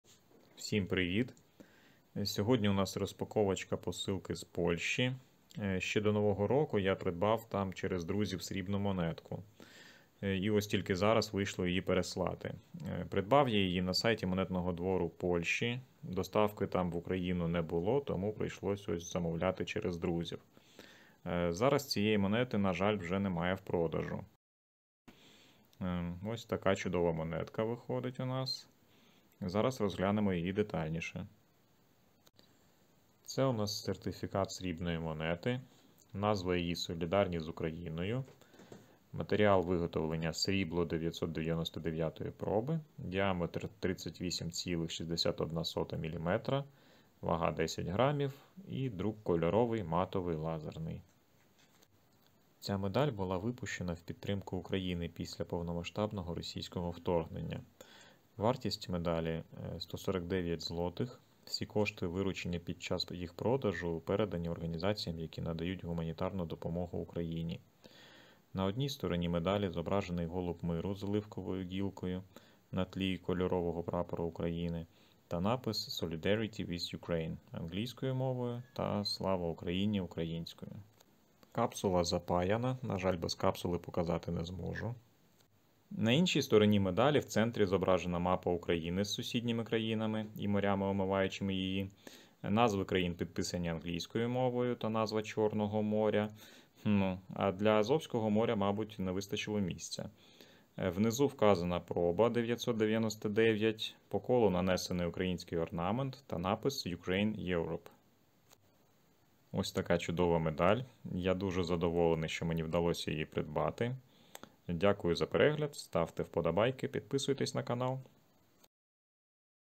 Срібна медаль "Cолідарні з Україною", "SOLIDARITY WITH UKRAINE", монетний двір Польші
Пам'ятна медаль випущена Польщею в підтримку України, всі кошти від продажу цієї медалі пішли на гуманітарну допомогу Україні.